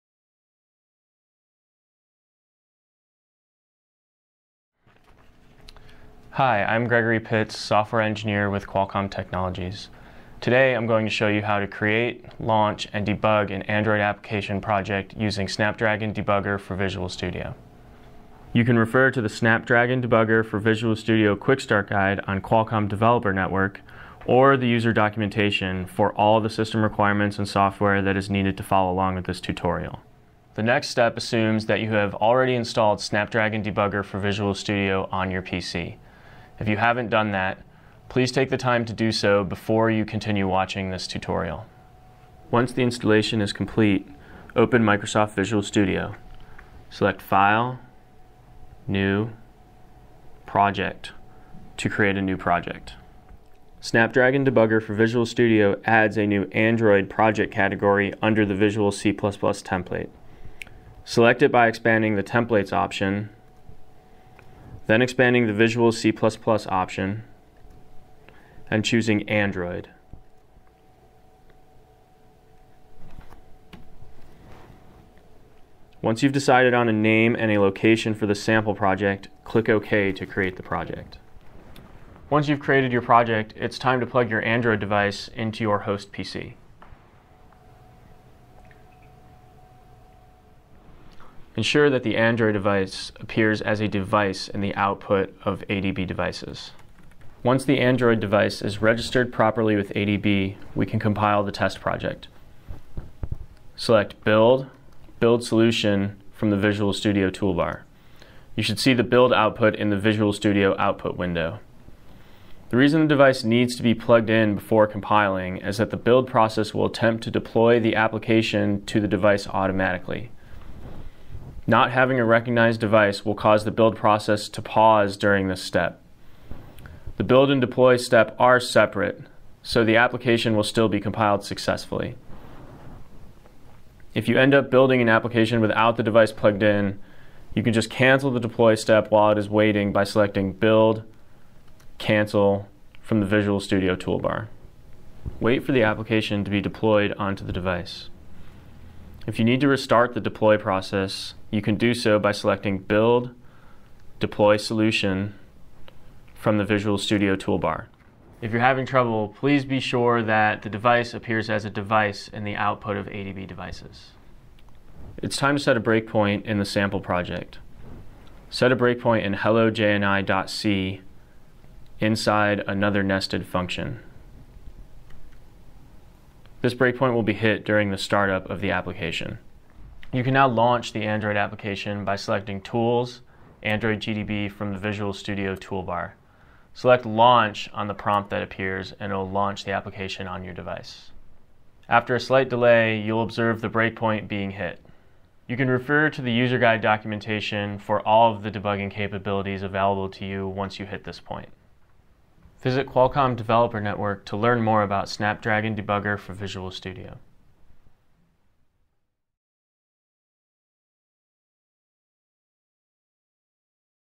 Snapdragon Debugger for Visual Studio Tutorial
This video will walk you through the steps to create, build, and launch a Native Android Application using Snapdragon Debugger for Visual Studio. By the end, you will have an Application running on your Android device that is paused at a breakpoint, allowing you to use many powerful debugging features.